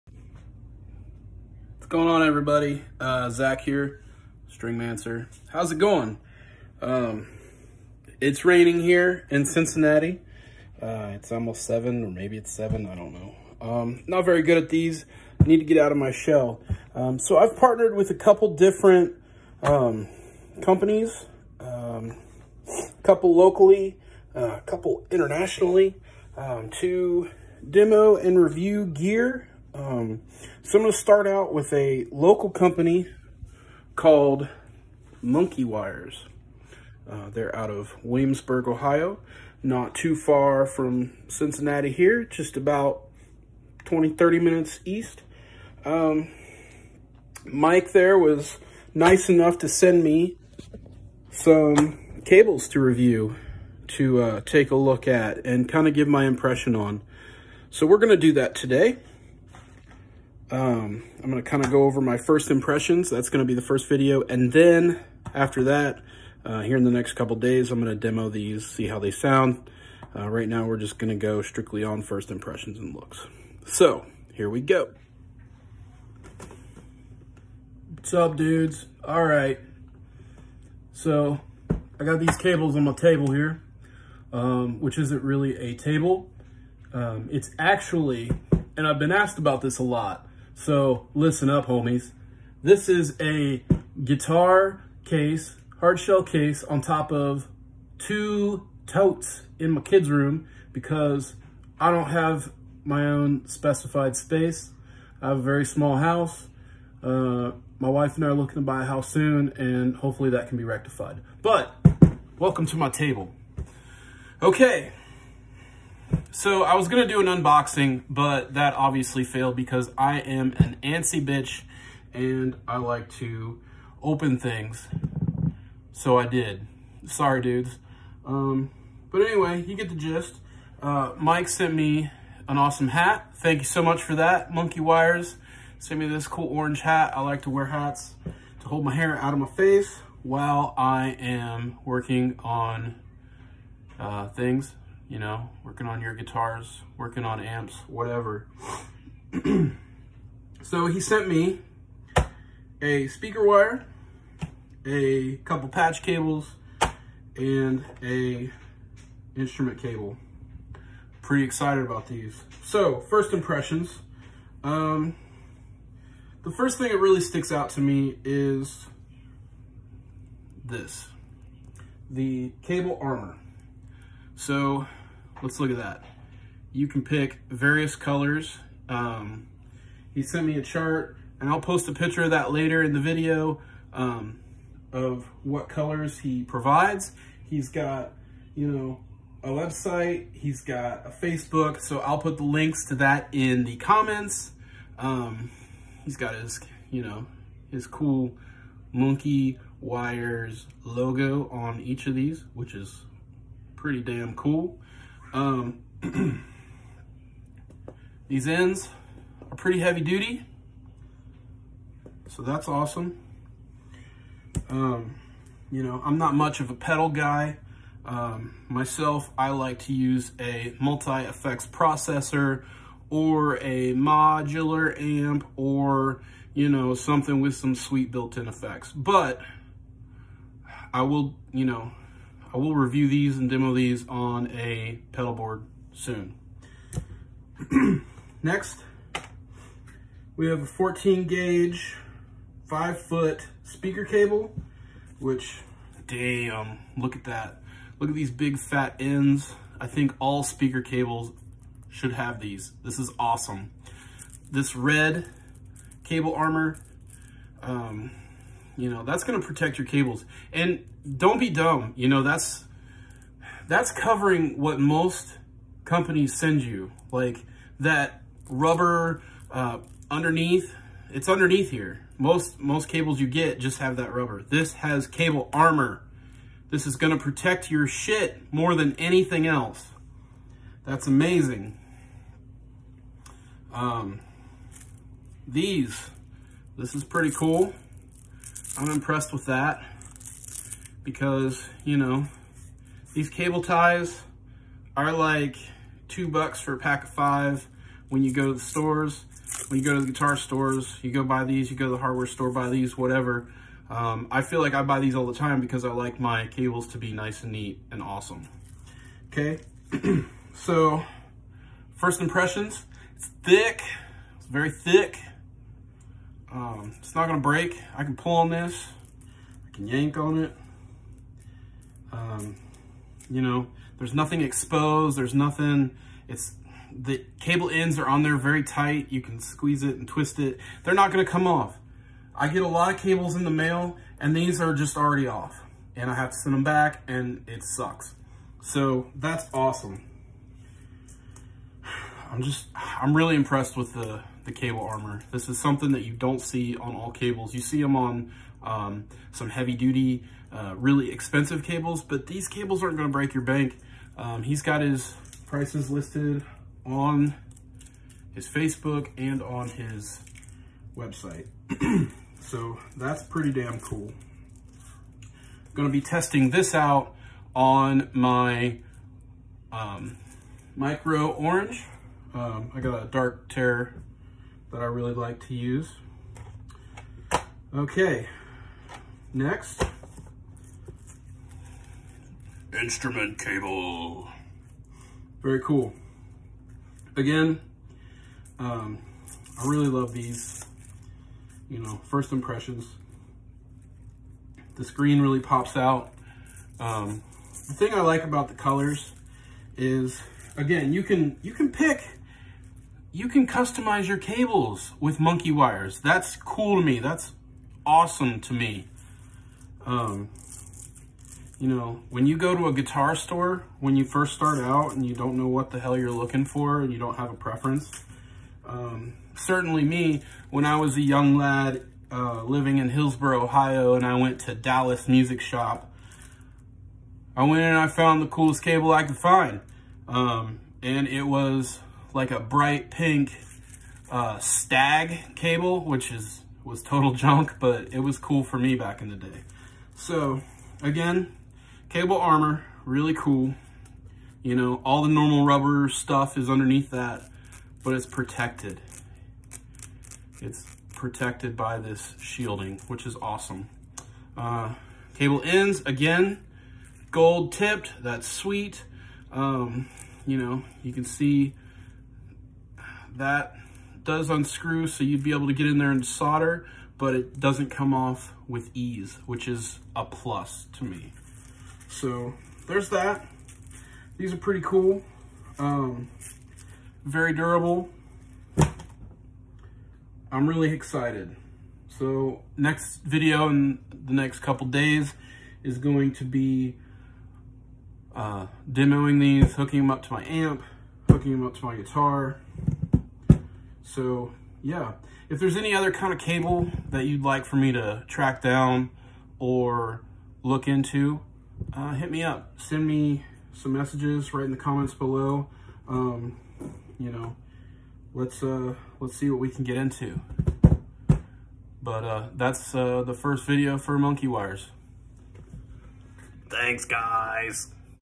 Monkey Wires FIRST IMPRESSIONS
First impressions video on Monkey Wires - instrument and speaker cables.